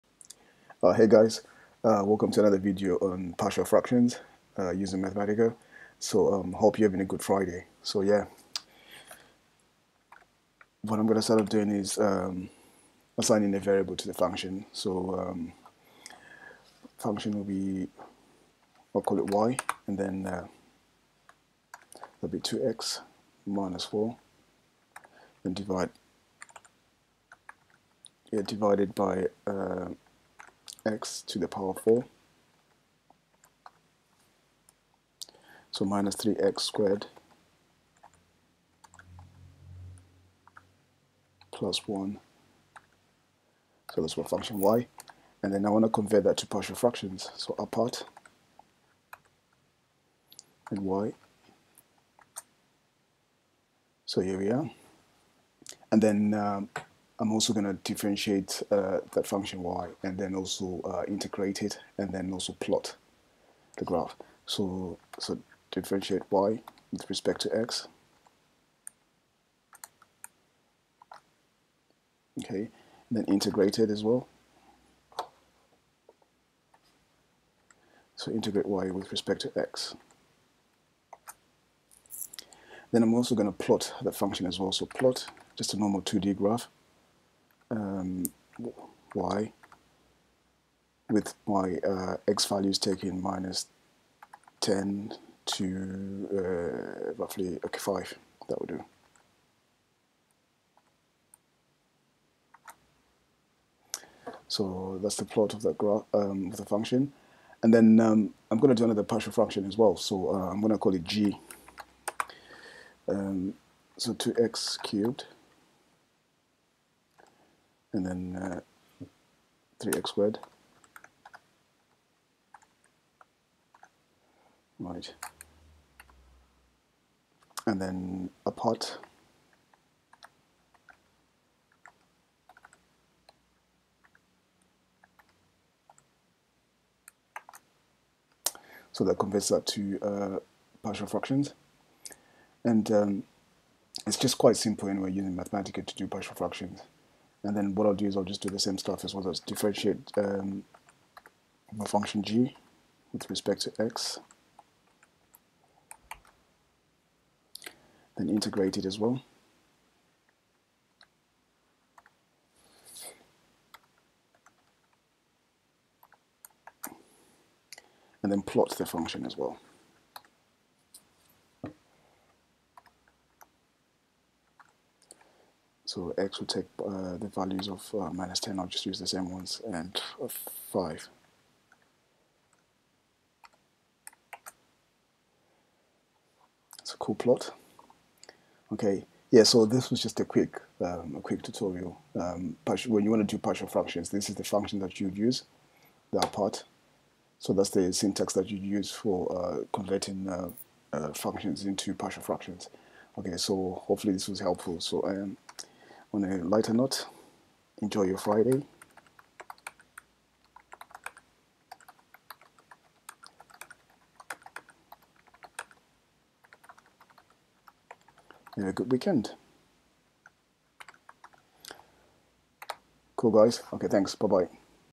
Partial Fraction Decomposition with Mathematica 12
Converting functions to partial fractions.
Differentiating, integrating, and plotting the functions.